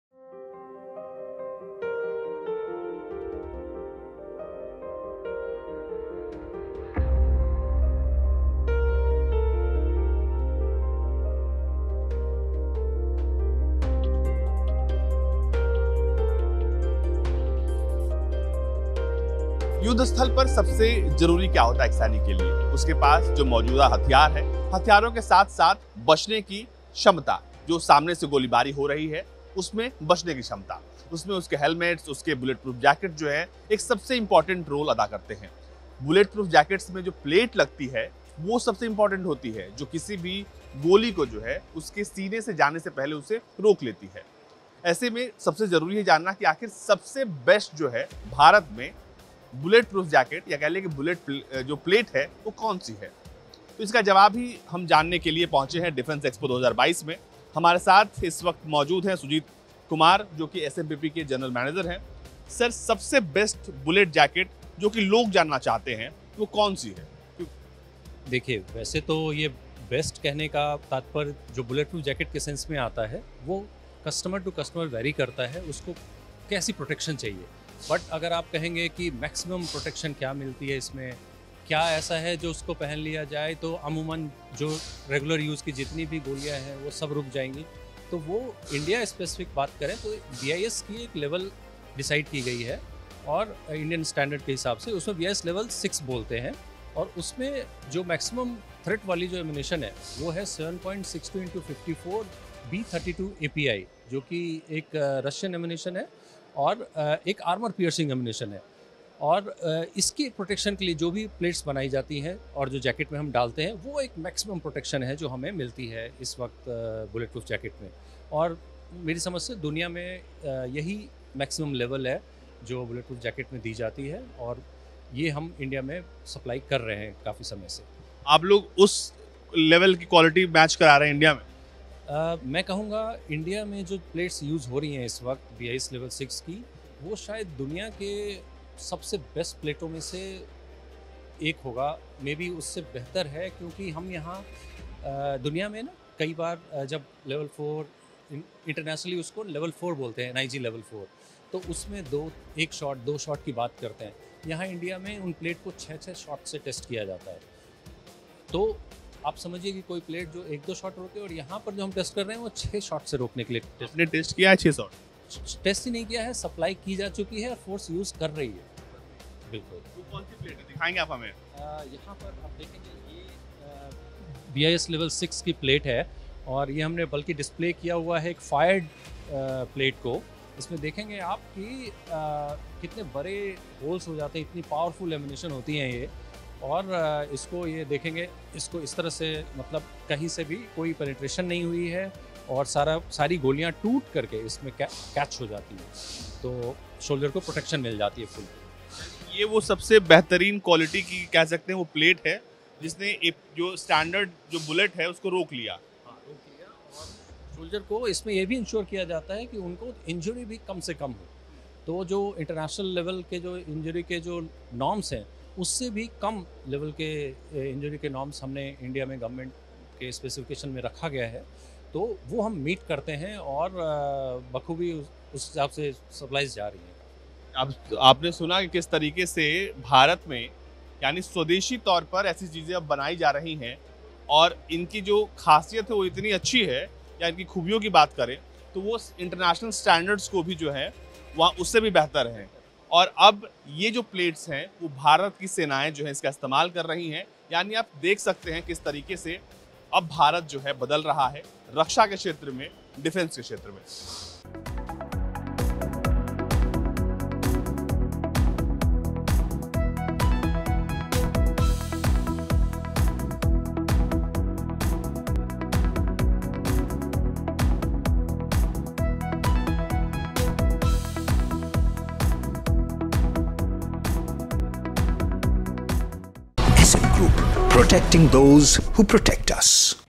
Advanced Armor Plate for Maximum Protection from SMPP DEFENSE
The latest Bullet Proof Jackets used by Indian Defense Forces provide maximum protection.